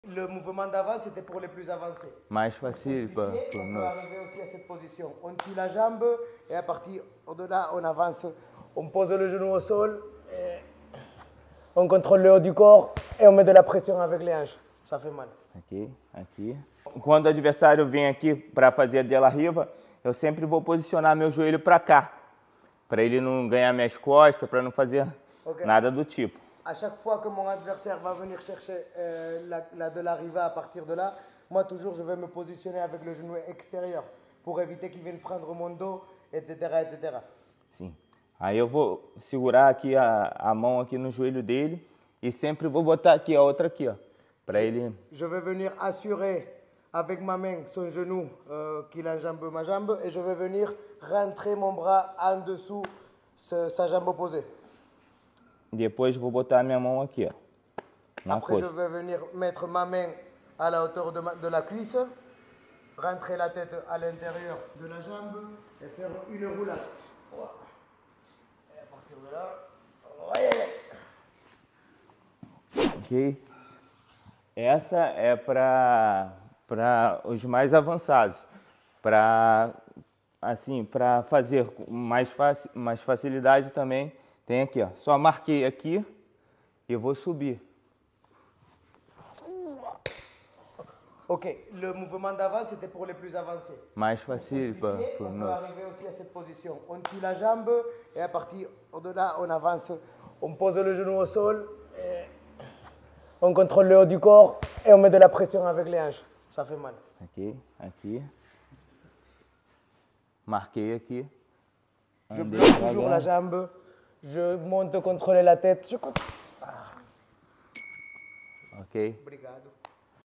Contrer la position DE LA RIVA | Technique de Luta Livre | Ricardo Cardoso
Rejoignez notre chaîne privée pour bénéficier d'avantages exclusifs
et de la programmation technique du NRFight Club Paris :